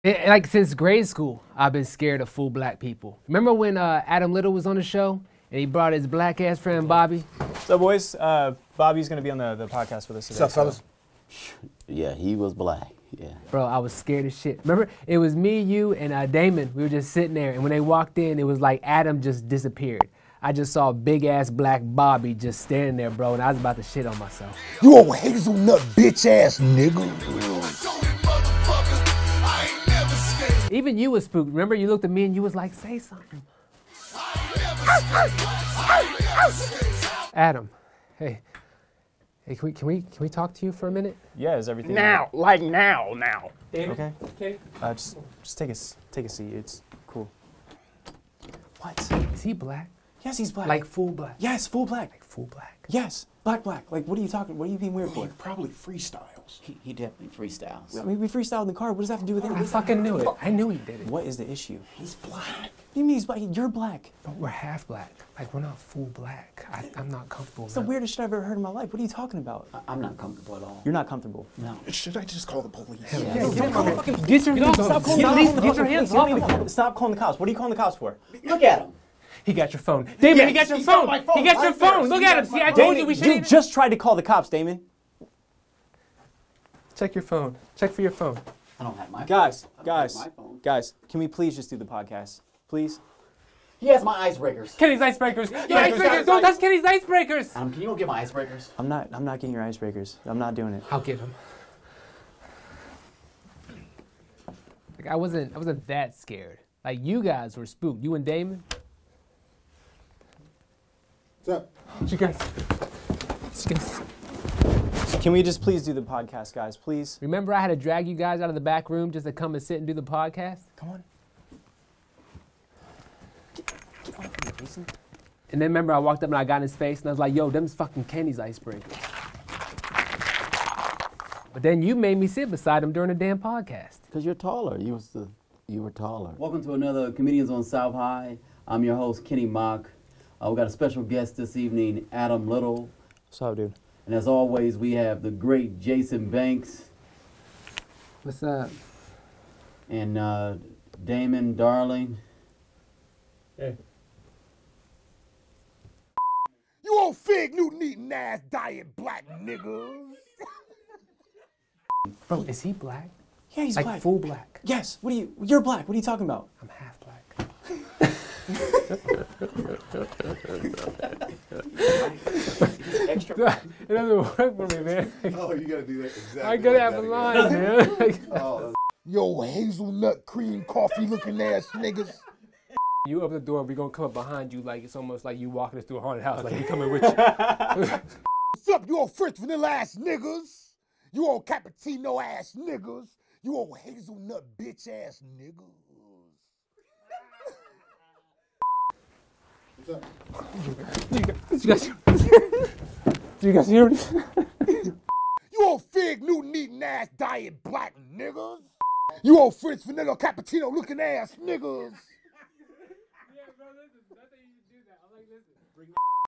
Scared of FULL BLACK people?
Apparently @jasonbankscomedy is scared of full black people...

Written by Jason Banks
Edited by Adam Little
Featuring Bobbie Dodds, Kenny Mock, Daymond Darling
Camera Men: Jared Bilinsky, Dusty Estep